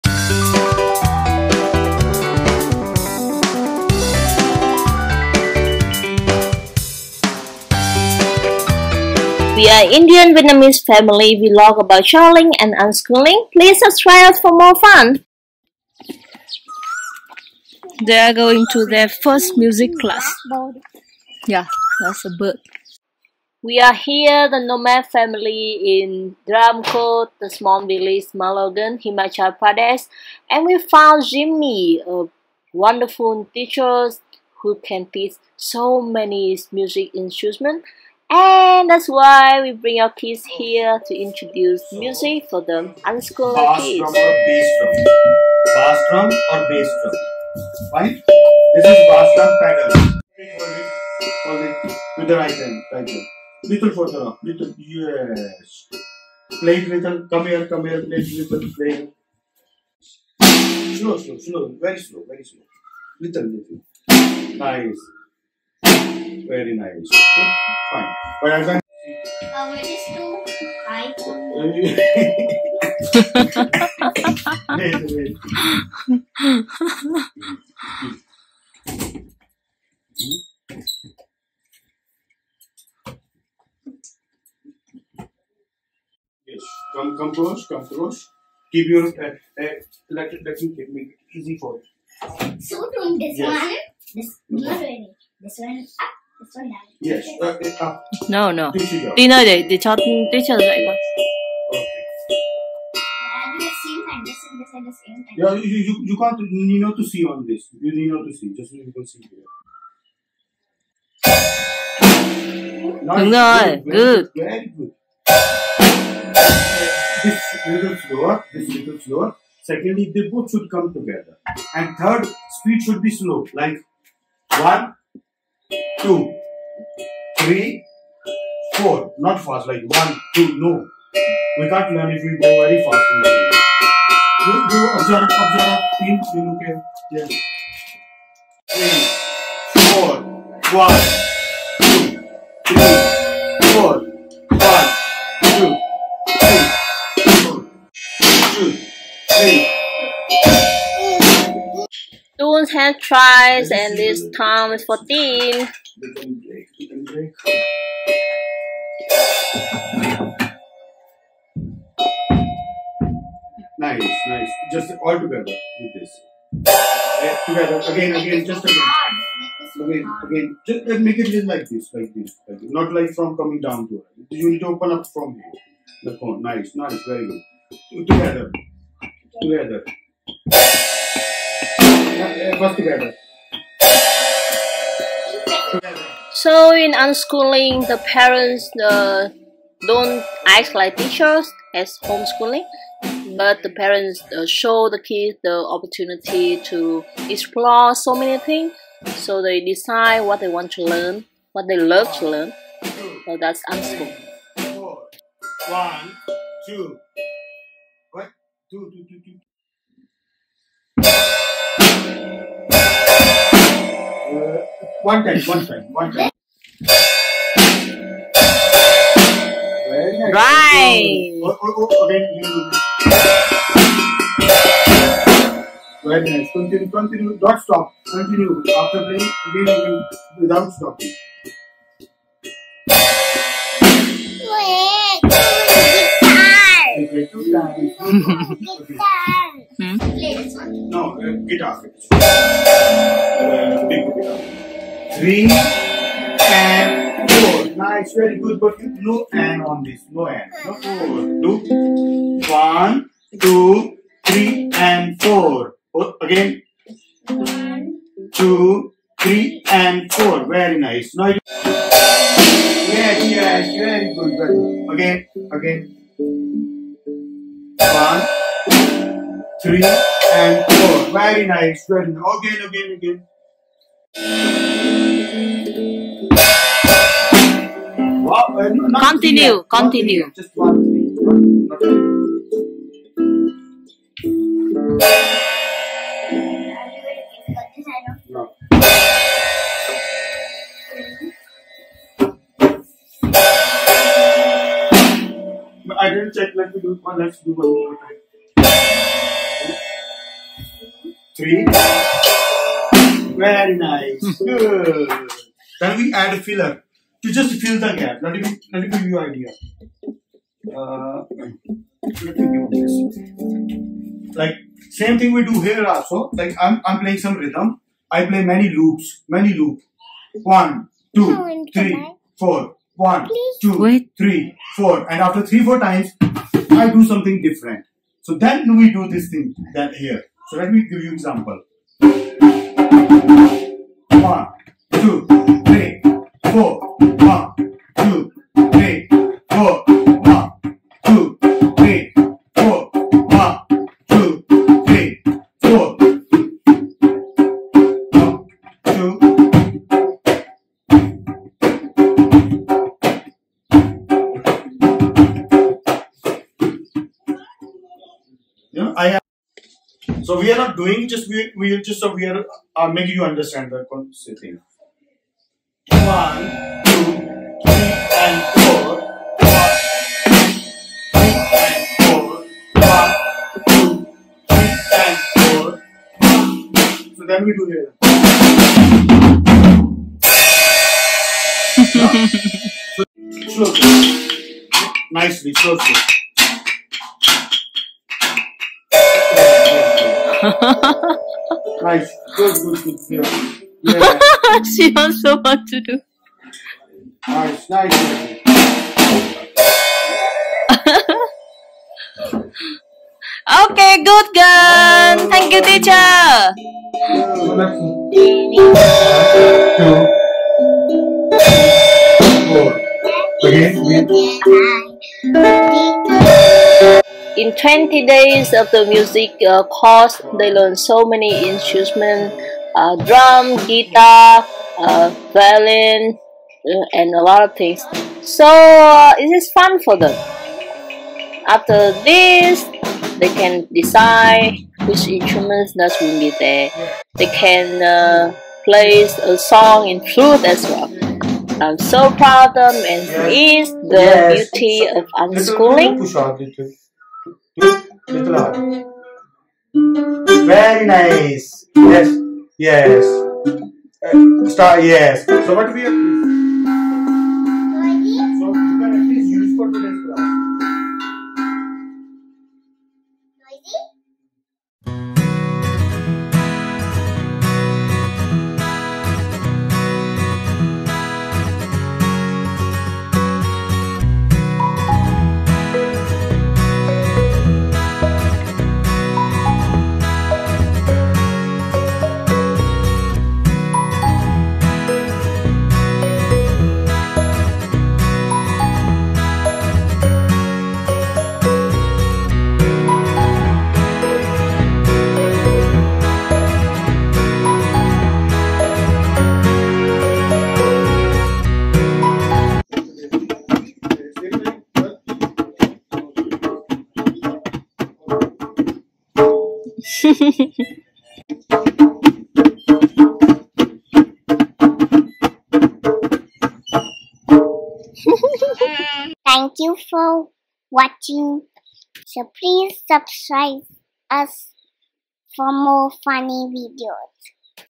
How To Introduce UNSCHOOLERS To Music - Ultimate Music Teacher in Himachal
How to unschool or does unschooling work? Yes, it works! We don't need to go to school to learn, but unschooling kids learn from everywhere and everything!
We are in the trip to Dharamkot, Himachal Pradesh, India and we found an amazing music teacher who teaches many kinds if instruments!

And it is the great chance to introduce music to our children. They got to know how to play a lot of different musical instrument. And as a part of unschooling journey, our little unschoolers will choose which is the best for them and what they want to learn deeper later!

We are Indian Vietnamese family of 5 who unschool and travel around the world.